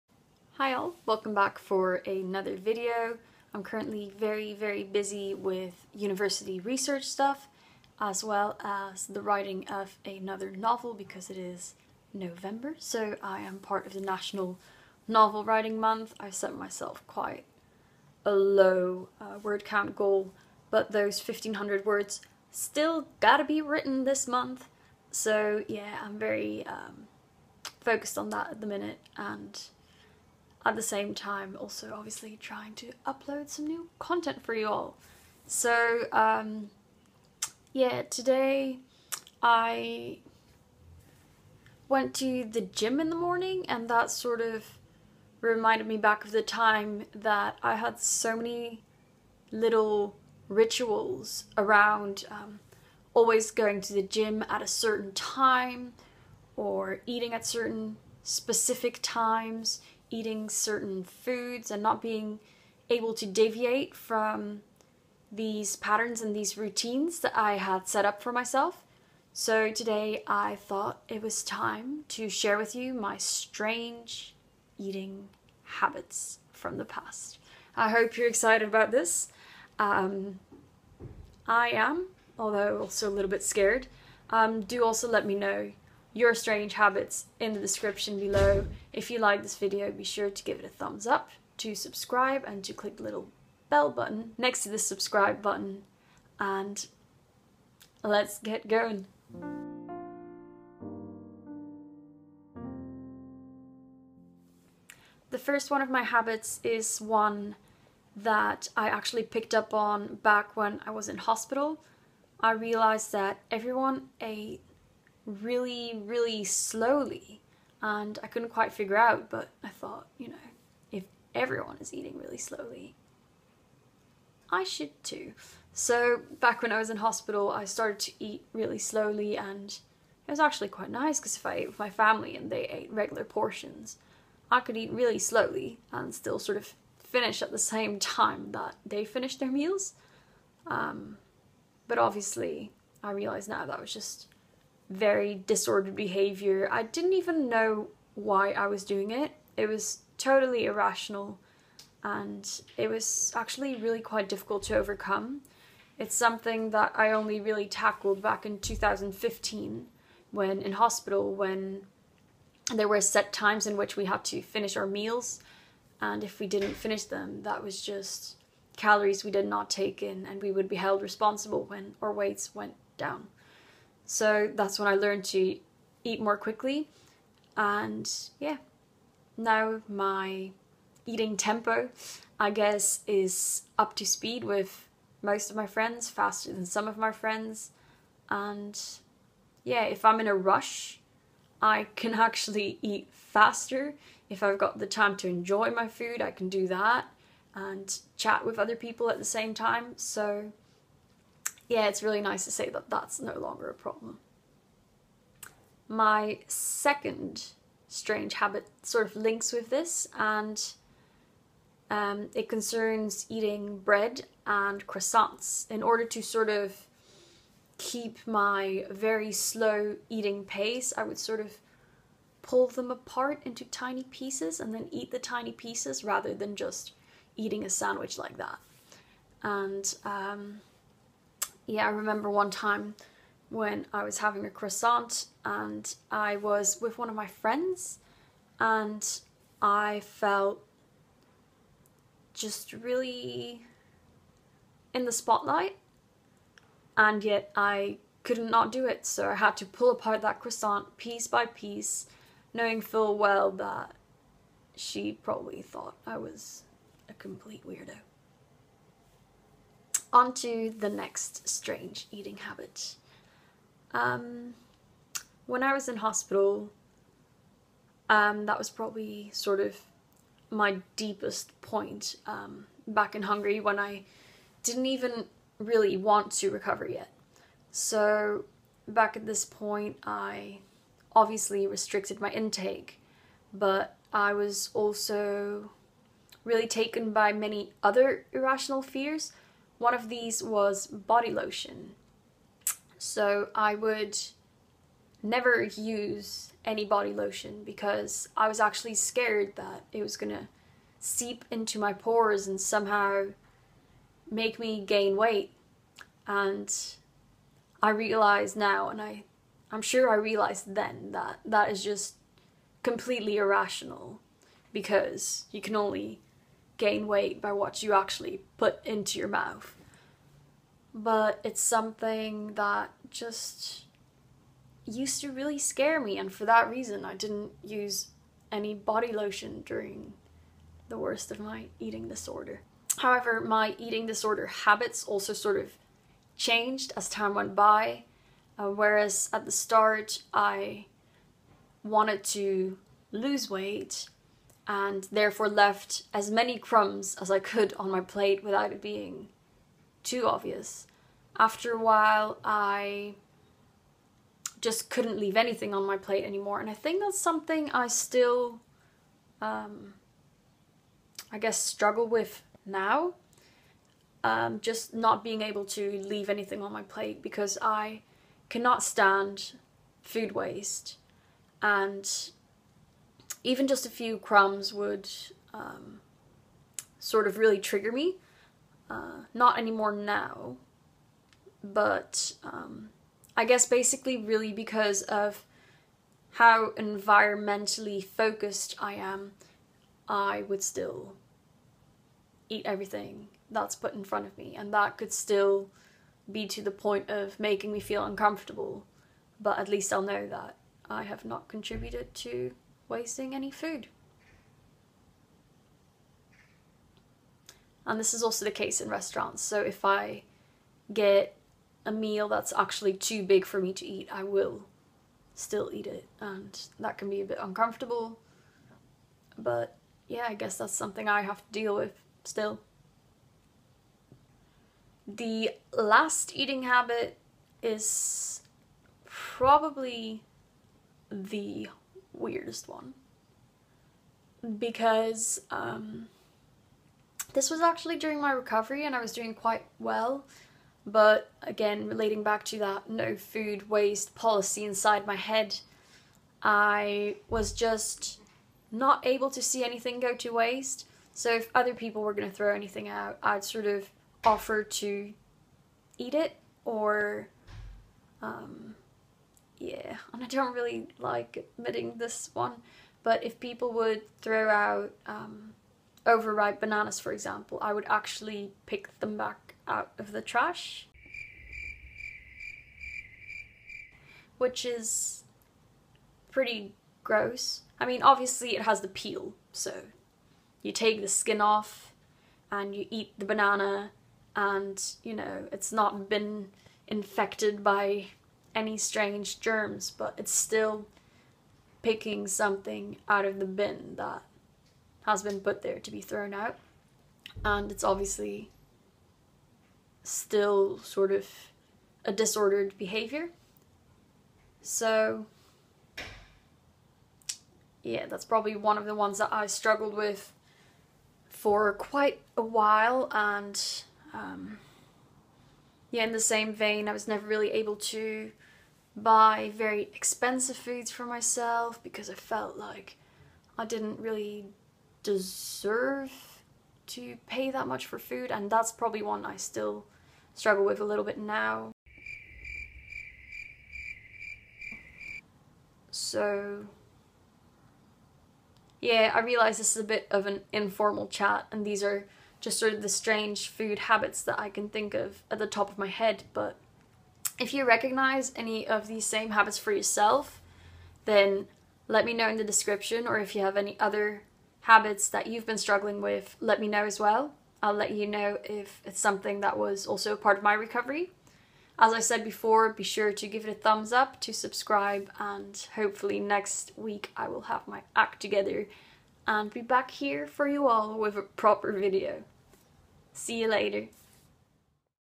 My Strange Eating Habits | Anorexia Recovery
Eating disorders change you, so it is no surprise that they also influence your habits. For me, anorexia did just that... in so many strange (and somewhat embarrassing) ways...

In fact, many of the ways that I felt personally impacted by my eating disorder are things I share candidly in my fictional memoirs, Just Perfect and Red Ribbons (links below). As mentioned, I am also working on a third book, which will chronicle some more personal experiences and difficult-to-uncover realities.

📚 BOOKS
Just Perfect
Red Ribbons